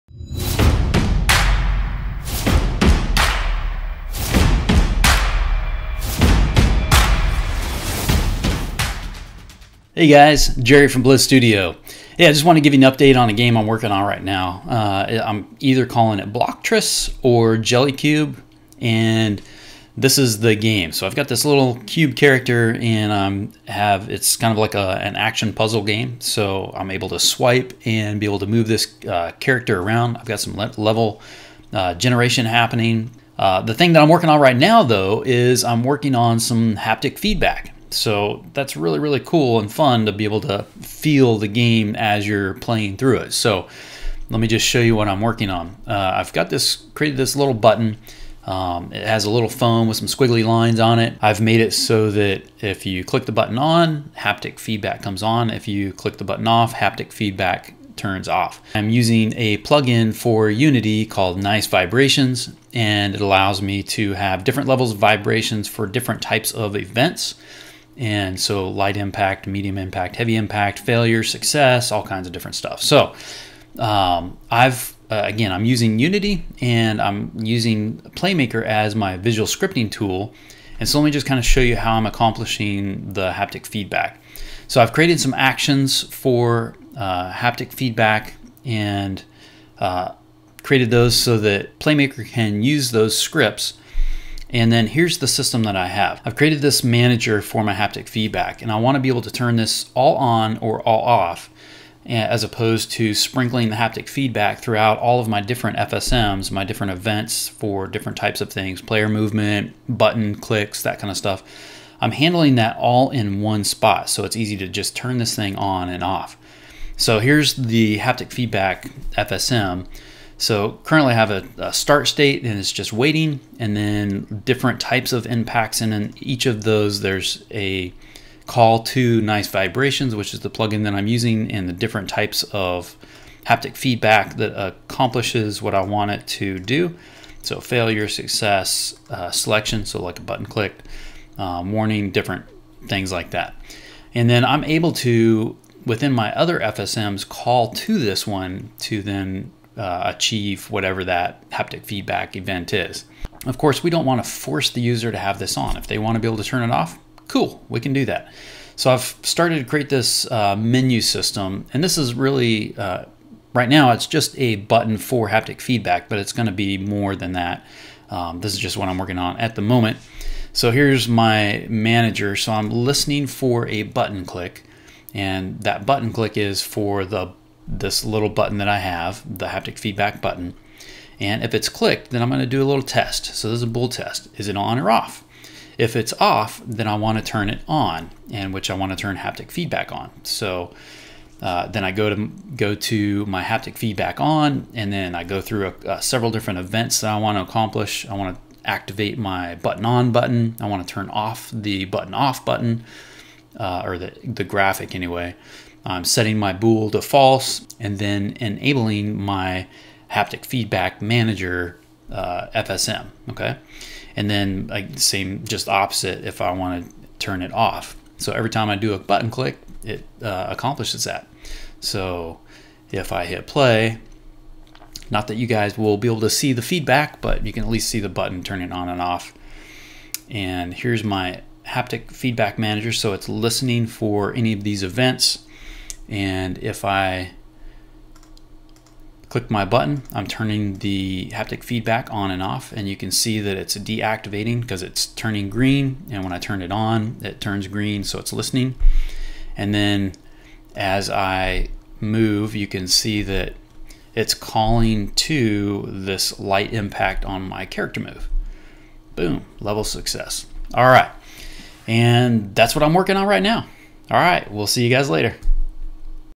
Devlog 01 - JellyCube - New Game setting up Haptic Feedback
I am creating a new mobile action puzzle game. I am using Unity with Playmaker as the visual scripting tool. In this devlog I’ll show how I am setting up haptic feedback and being able to turn it all on or all off.